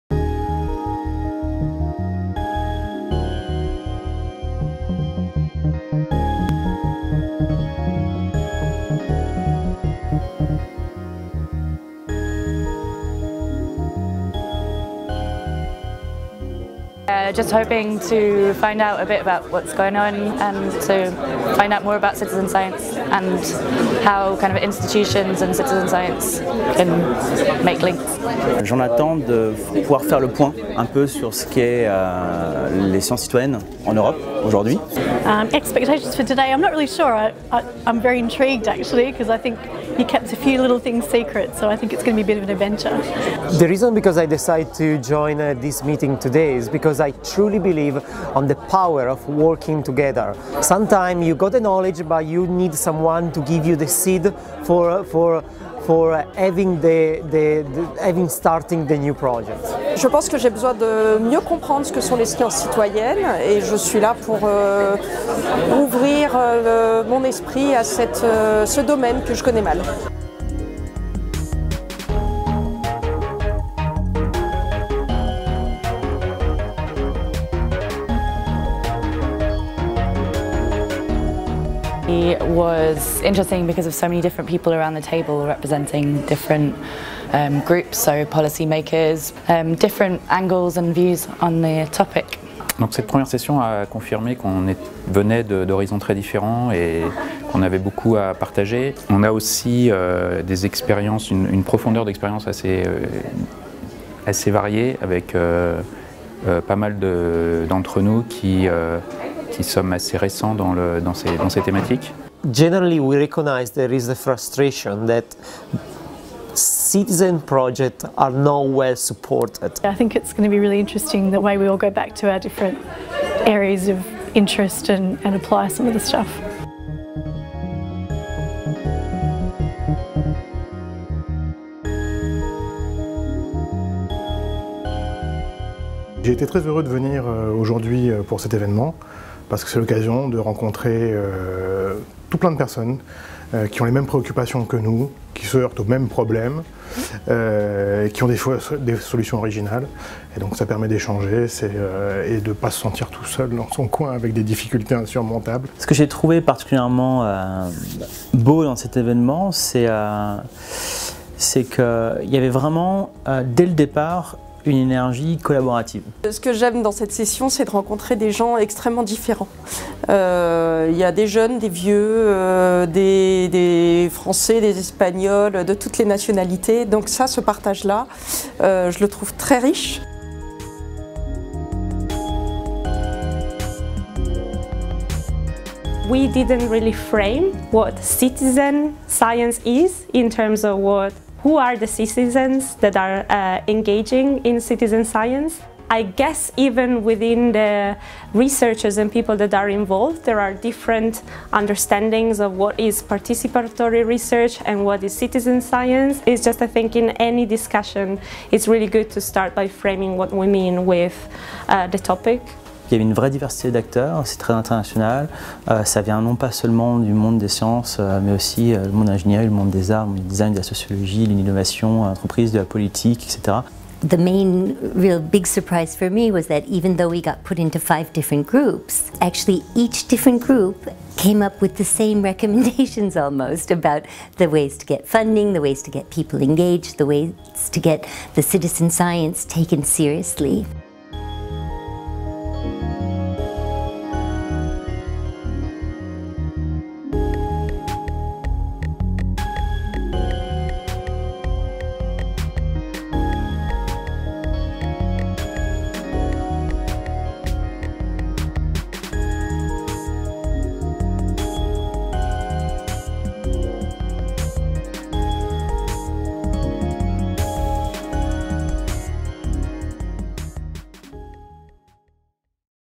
DITOS - Citizen science & DIY Bio : building the bridge between citizens and institutions
European Citizen Science Forum
Paris, March 25th 2017